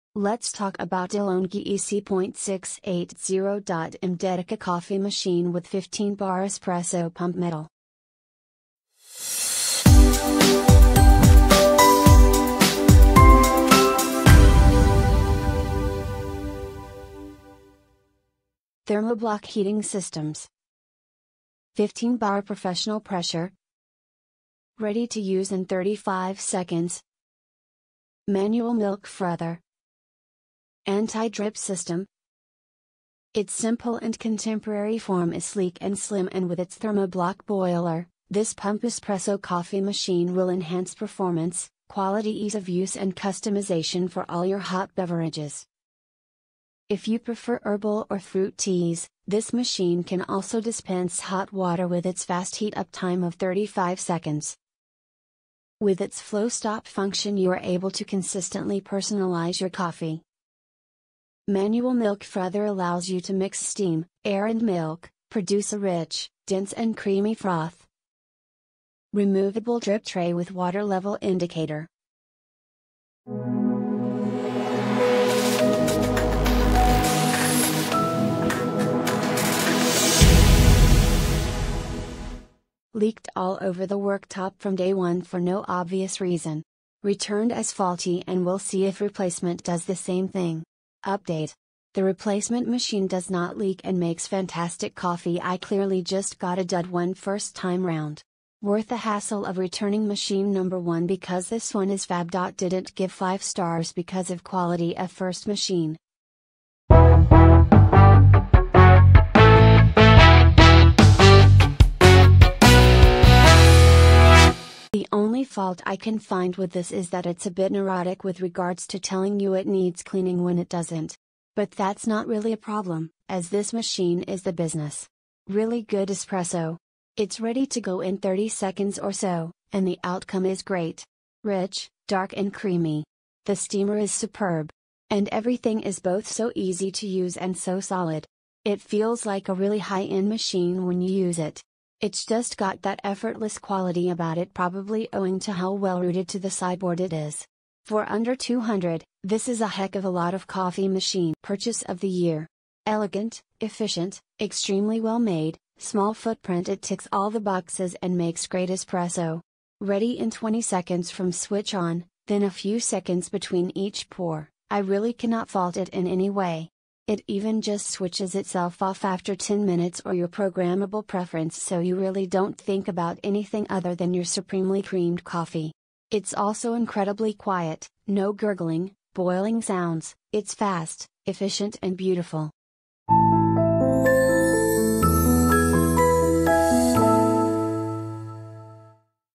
Review De'Longhi EC.680.M Dedica Coffee Machine with 15 Bar Espresso Pump - M [2019]
Thermoblock heating systems

15 bar professional pressure

Ready to use in 35 seconds

Manual milk frother

Anti drip system

Its simple and contemporary form is sleek and slim and with its Thermoblock boiler, this pump espresso coffee machine will enhance performance, quality ease of use and customisation for all your hot beverages

If you prefer herbal or fruit teas, this machine can also dispense hot water with its fast heat-up time of 35 seconds

With its flow stop function you are able to consistently personalise your coffee

Manual milk frother allows you to mix steam, air and milk, produce a rich, dense and creamy froth

Removable drip tray with water level indicator